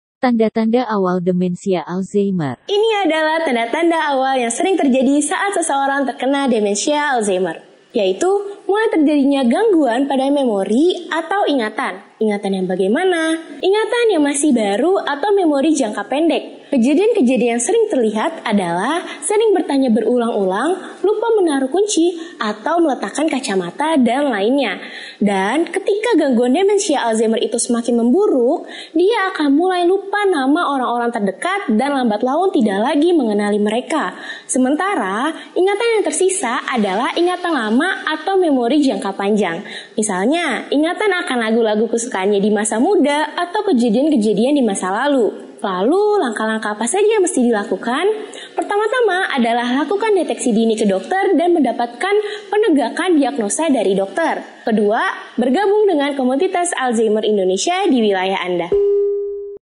Tanda-Tanda Awal Demensia Alzheimer
Tanda-Tanda Awal Terkena Demensia Alzheimer.
Ini adalah tanda-tanda awal yang sering terjadi saat seseorang terkena Demensia Alzheimer…
Yaitu mulai terjadinya gangguan pada memori atau ingatan.
Ingatan yang bagaimana? Ingatan yang masih baru atau memori jangka pendek.
Kejadian-kejadian yang sering terlihat adalah…

Sering bertanya berulang-ulang, lupa menaruh kunci, lupa meletakkan kacamata dll.
Ketika gangguan Demensia Alzheimer itu makin memburuk,
dia akan mulai lupa nama-nama orang terdekat dan lambat laun tidak lagi mengenali mereka.
Sementara, ingatan yang tersisa adalah ingatan lama atau memori jangka panjang.
Misalnya ingatan akan lagu-lagu kesukaannya di masa muda, atau kejadian-kejadian di masa lalu.

Lalu, langkah apa saja yang mesti dilakukan?
Pertama, adalah lakukan deteksi dini ke dokter dan mendapatkan penegakan diagnosa.
Kedua, bergabung dengan komunitas Alzheimer Indonesia di wilayah Anda.